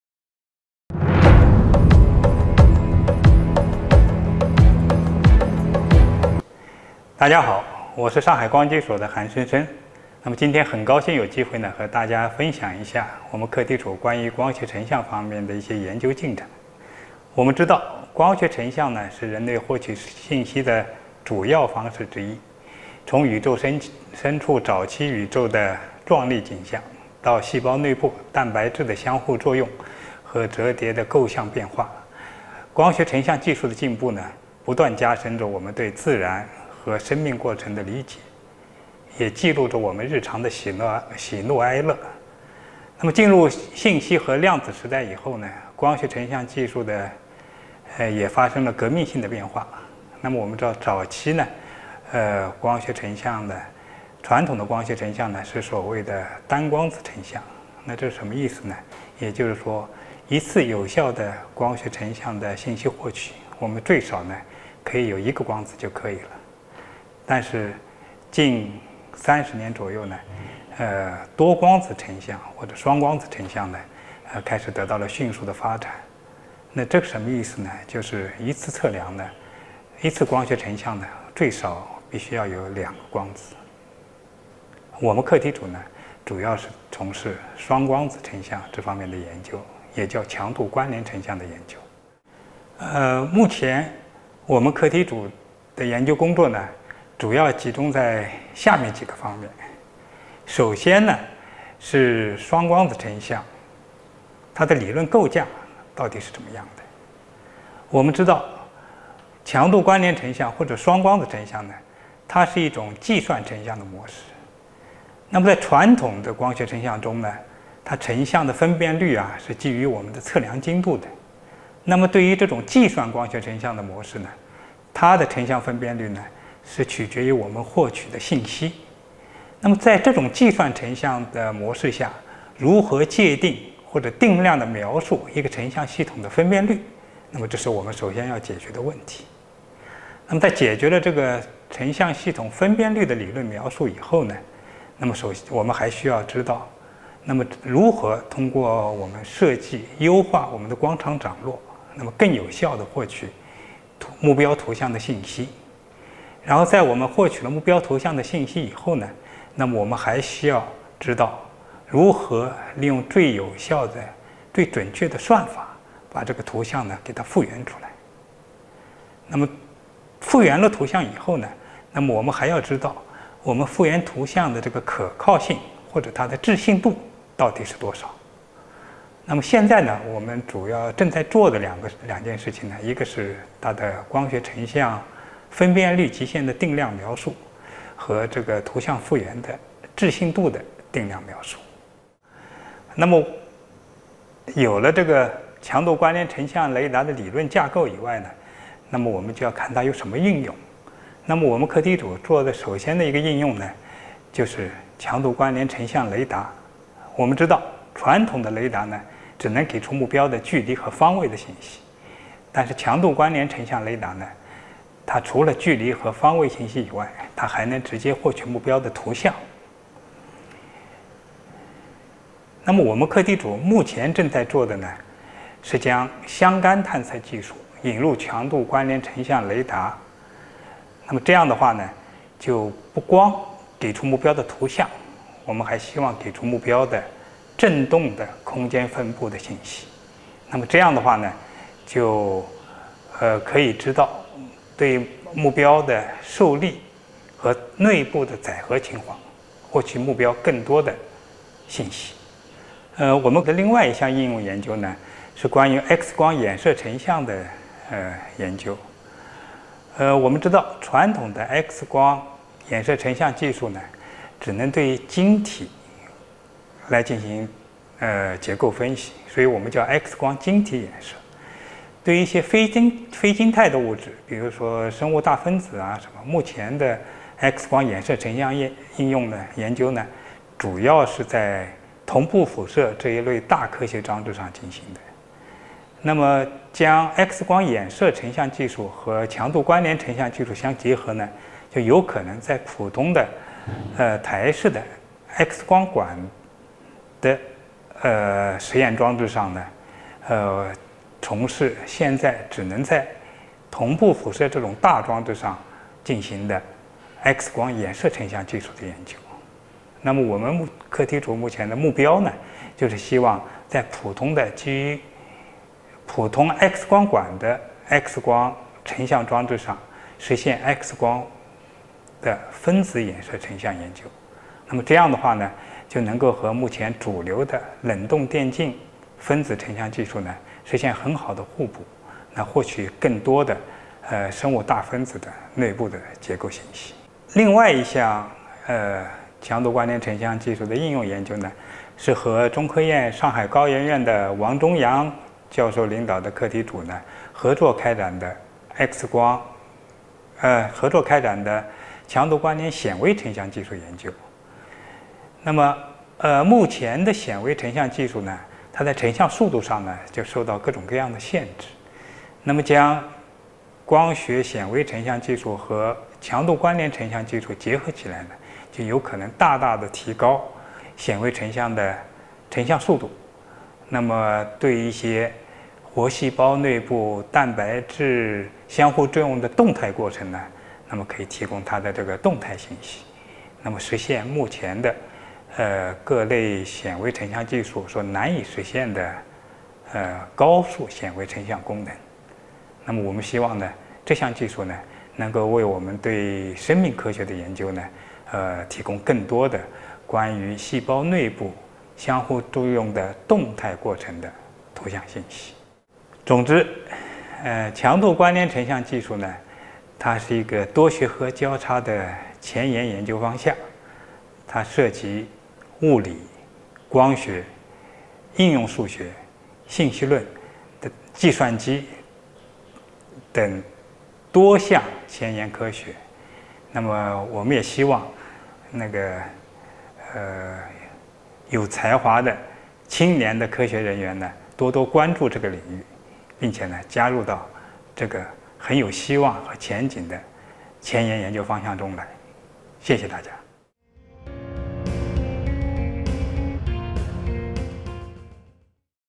五分钟光学: 上海光机所韩申生研究员谈课题组光学成像研究进展
光学成像是人类获取信息的主要方式之一，从宇宙的壮丽景象到细胞内部蛋白质的相互作用、构象变化，光学成像技术的进步不断加深着我们对自然和生命过程的理解。进入信息和量子时代以后，多光子成像（双光子成像）技术得到了迅速的发展，韩申生研究员课题组对双光子成像（也称强度关联成像）的研究集中在两大方面：
1）双光子成像的理论构架，包括光学成像分辨率极限的定量描述和图像复原置信度的定量描述。
2）成像技术的应用研究，包括强度关联成像雷达、X光衍射成像、强度关联显微成像技术。

Optical imaging is one of the main ways for humans to obtain information. From the magnificent scenery of the universe to the interaction and conformational changes of proteins inside cells, the advancement of optical imaging technology continuously deepens our understanding of natural and life processes. Since entering the information and quantum era, multi-photon imaging (such as two-photon imaging) technology has rapidly developed. Dr. Han Shen-Sheng's research group focuses on two aspects of two-photon imaging (also known as intensity correlation imaging):
1) The theoretical framework of two-photon imaging, including the quantitative description of the resolution limit of optical imaging and the quantitative description of confidence in image restoration.
2) The applied research of imaging technology, including intensity correlation imaging radar, X-ray diffraction imaging, and intensity correlation microscopy imaging technology.